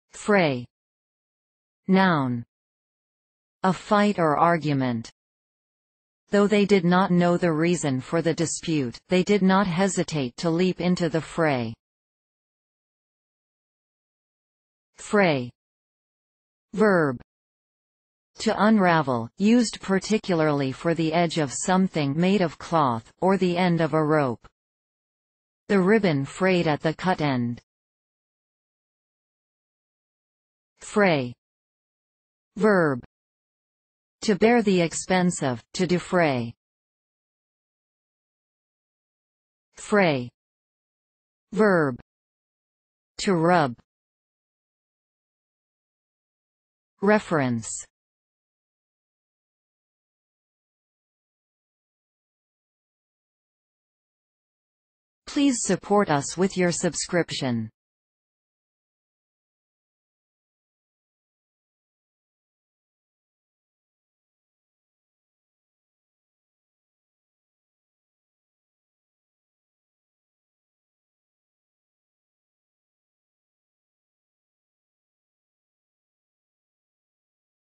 Fray | Definition of fray
See here, the definitions of the word fray, as video and text.

fray (noun)
    A fight or argument.
        Though they did not know the reason for the dispute, they did not hesitate to leap into the fray.

fray (verb)
    To unravel; used particularly for the edge of something made of cloth, or the end of a rope.
        The ribbon frayed at the cut end.

fray (verb)
    To bear the expense of; to defray.

fray (verb)
    To rub.

Reference:
    fray